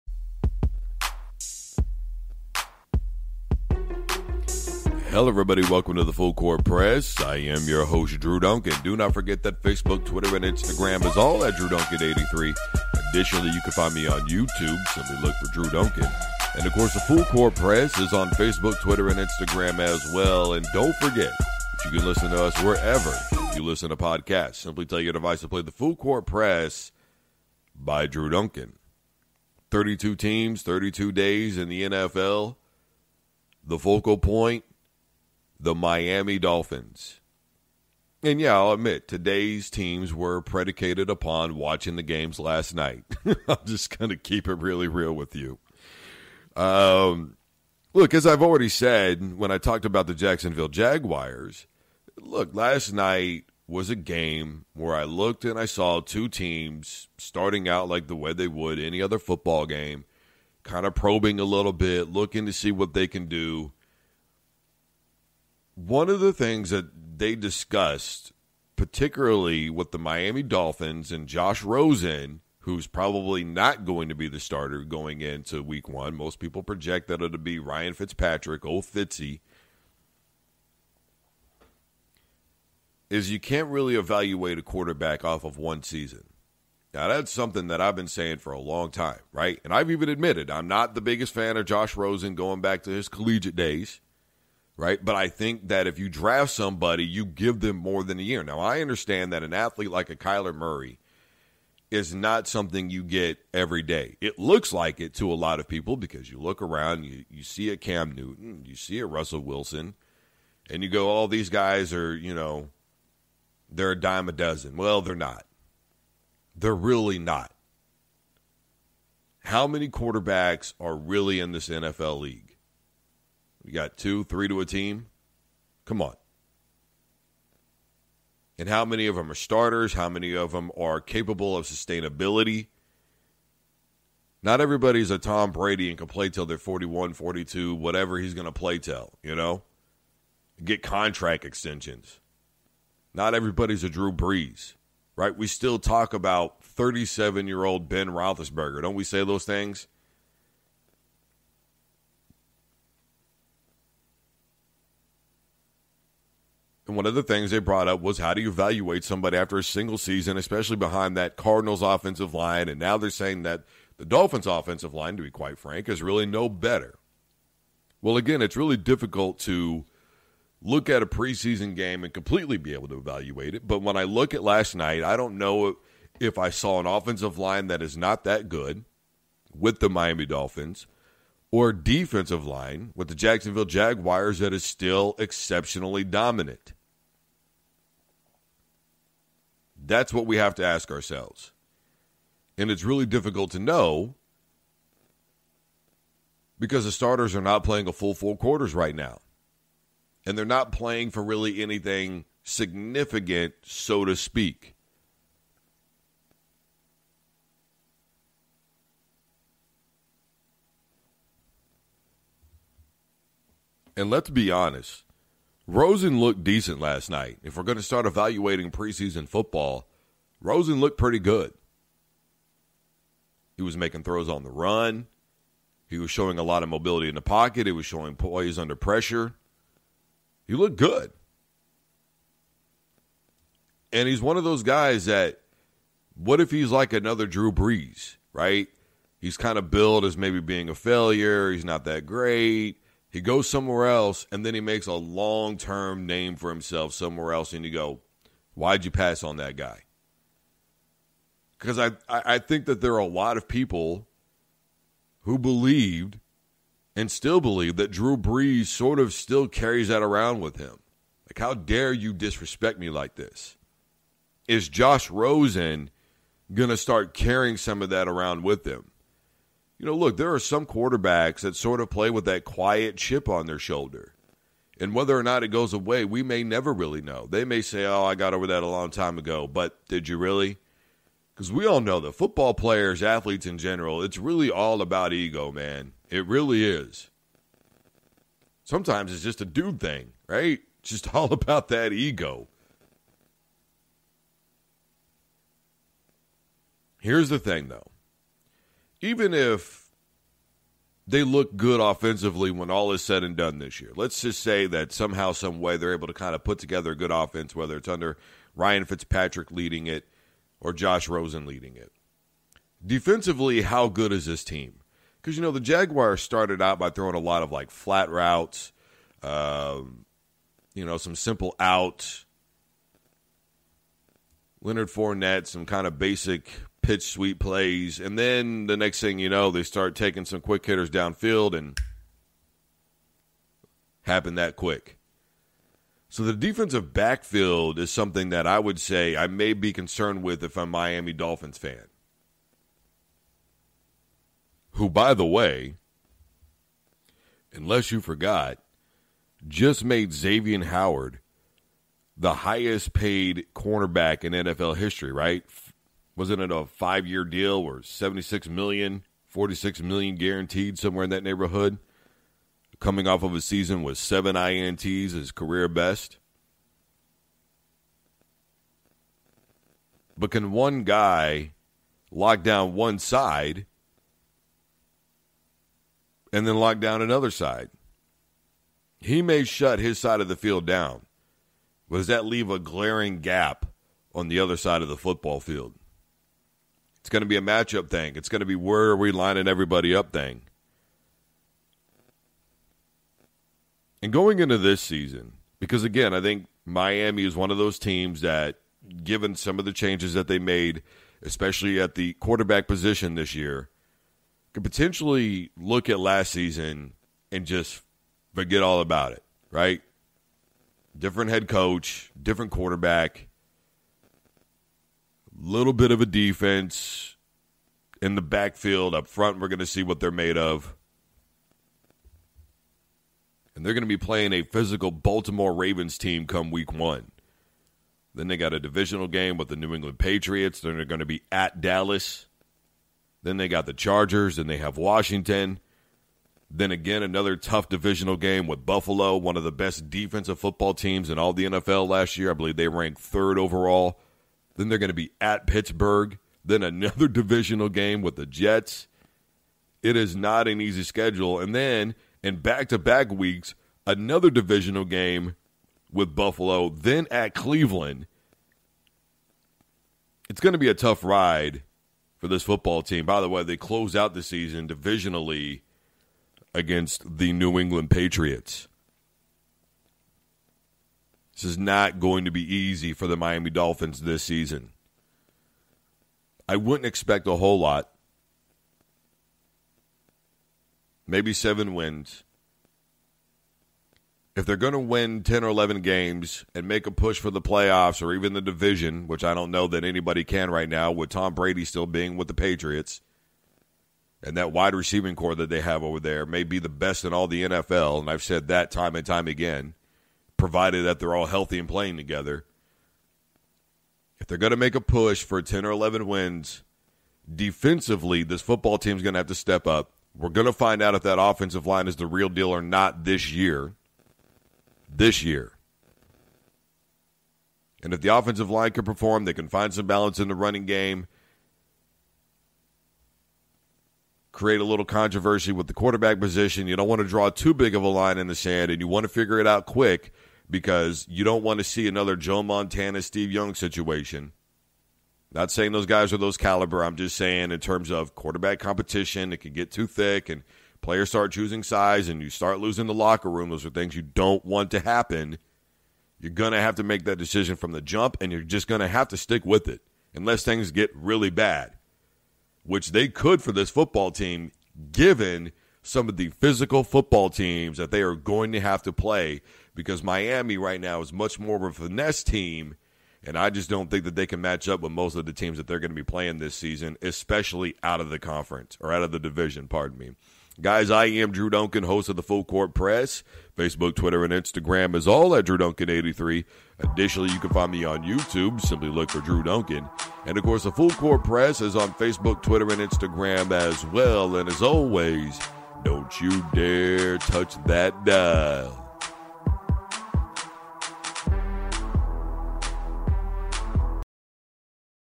A Look At The Dolphins Upcoming Season (made with Spreaker)
Who will be the starting quarterback come week 1? Will Xavier Hampton, live up to the money? Can the offensive line protect their quarterback? How will they do against more physical football teams?